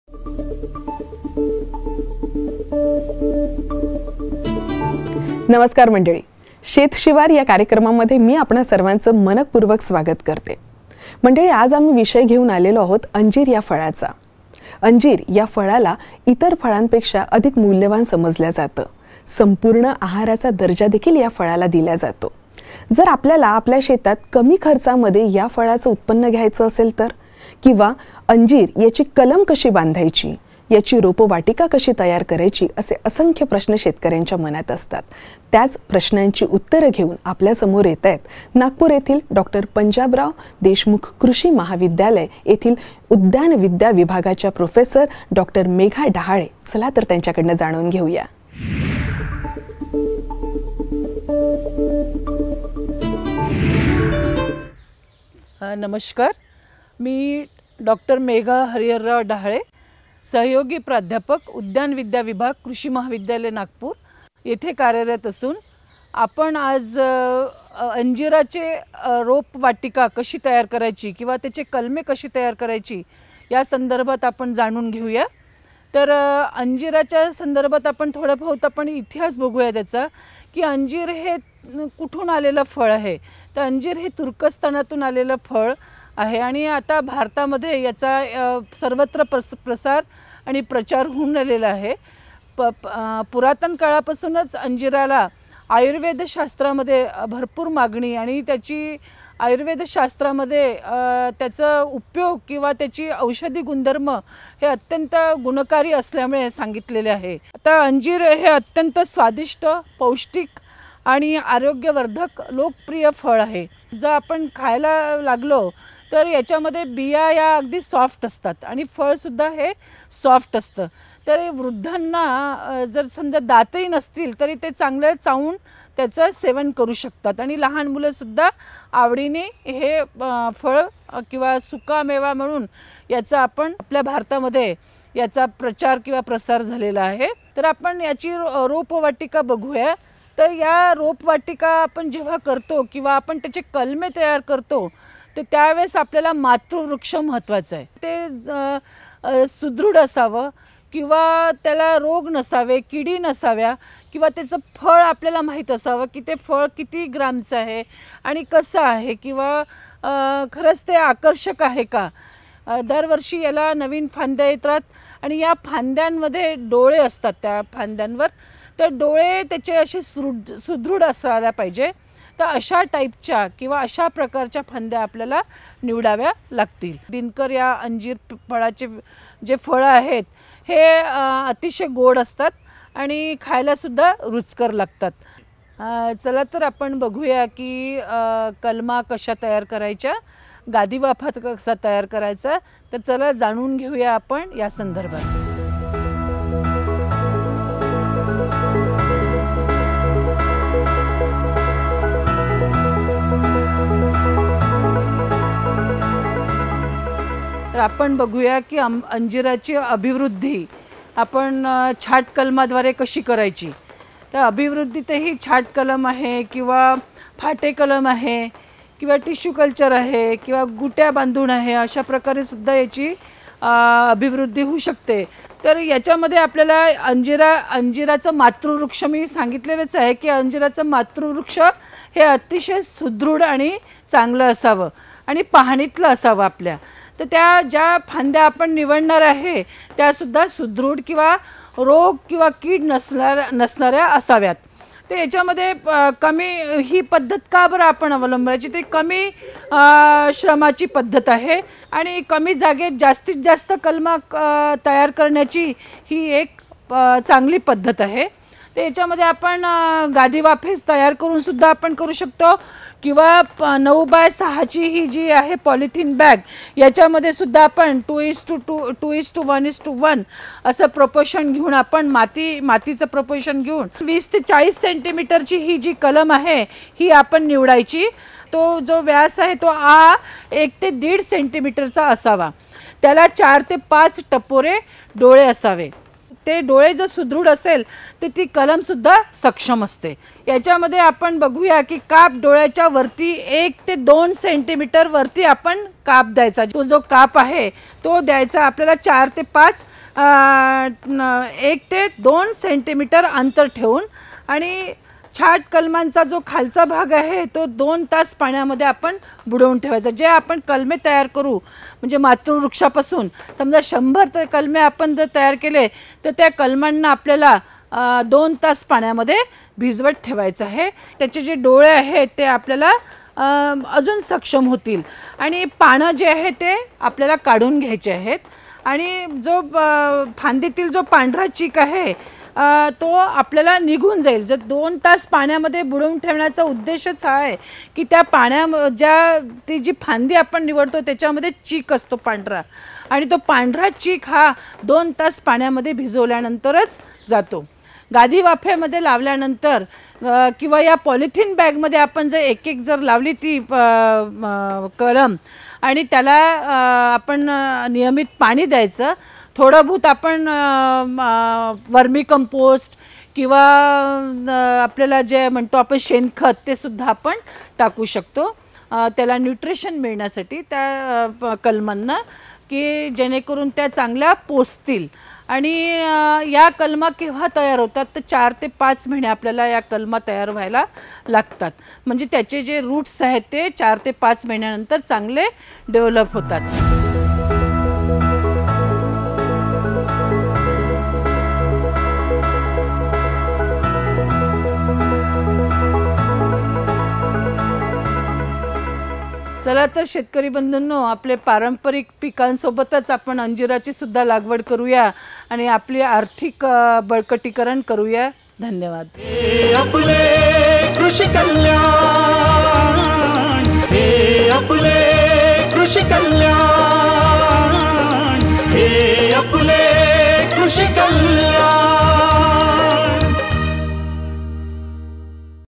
कृषी कल्याण । शेत शिवार
अंजीर रोपवाटिका

अंजिराचं उत्पादन अलीकडच्या काळात वाढतंय. विदर्भातील शेतकरी आता अंजीर उत्पादन घेऊ लागलेय. योग्य नियोजन आणि मार्गदर्शनाची साथ याला मिळाल्यास यातून कृषी उत्पन्न वाढविता येऊ शकतं. यासंदर्भात ‘शेत-शिवार’मध्ये मार्गदर्शन करत आहेत, नागपूर कृषी महाविद्यालयातील उद्यानविद्या विभागाच्या प्राध्यापक डॉ. मेघा डहाळे. जाणून घेऊया कमी खर्चात अंजीर लागवड कशी करायची ते...आपल्या ‘कृषी कल्याण’ यू-ट्युब चॅनलवर..